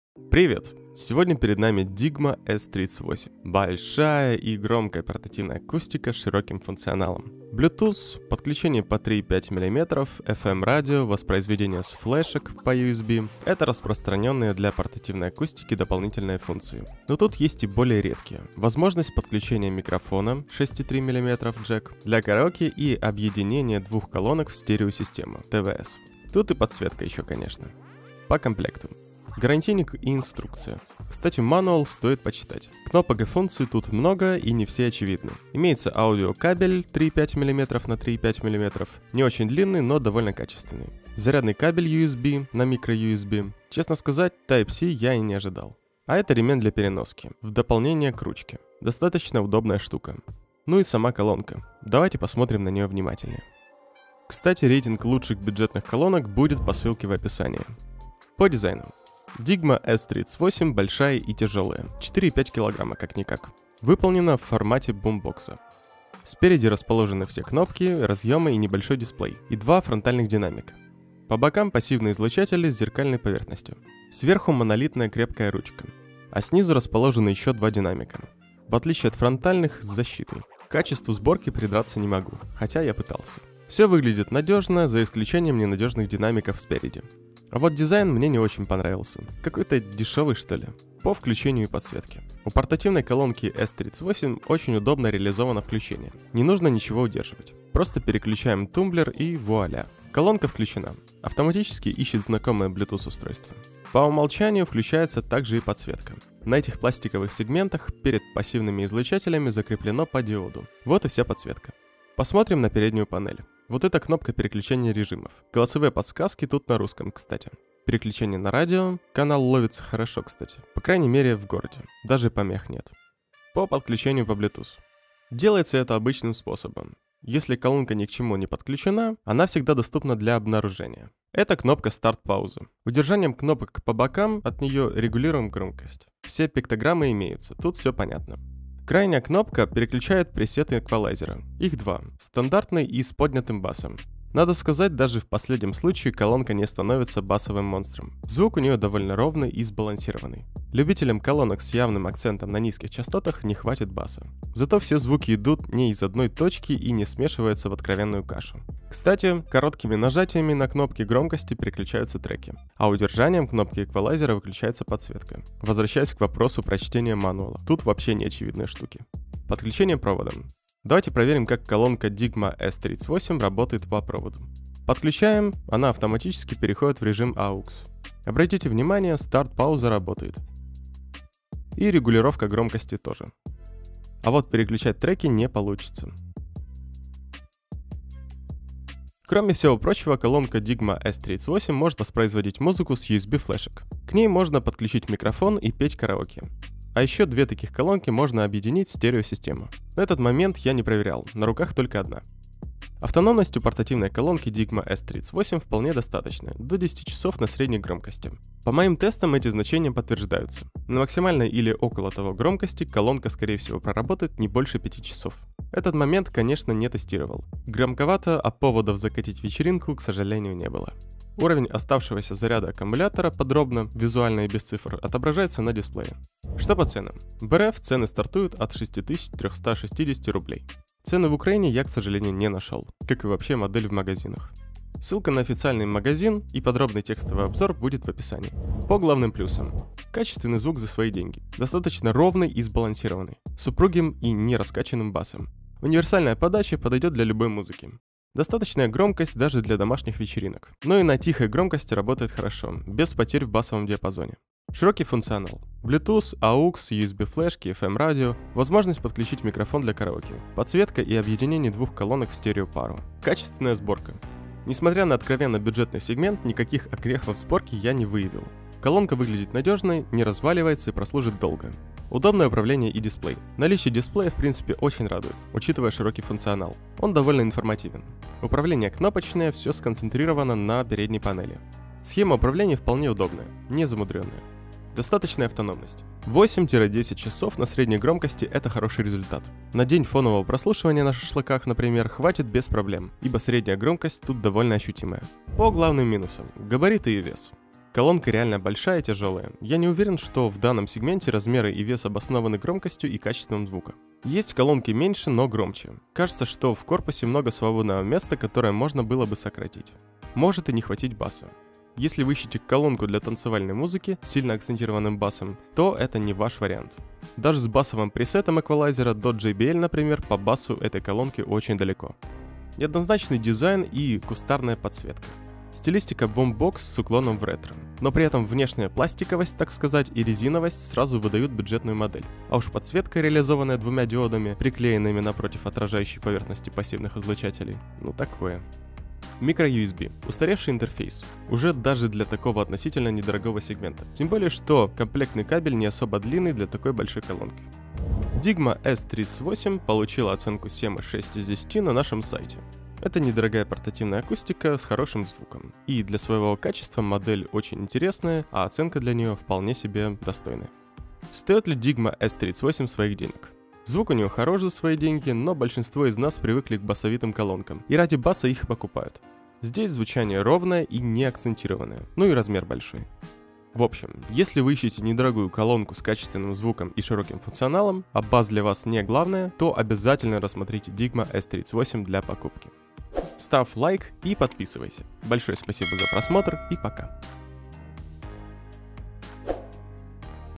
Digma S-38 - большая, громкая и недорогая колонка [ОБЗОР]
Digma S-38 – большая и громкая портативная акустика с широким функционалом. Bluetooth, 3,5 мм, FM-радио, воспроизведение с флешек (USB).

0:00 - Вступление
0:29 - Комплектация
0:52 - Внешний вид
1:31 - Управление и подключение
2:50 - Подключение проводом и функционал
3:21 - Автономность
3:48 - Цены
3:59 - Главные плюсы
4:55 - Главные минусы
5:48 - Итоговая оценка
6:00 - Стоит ли купить колонку Digma S-38?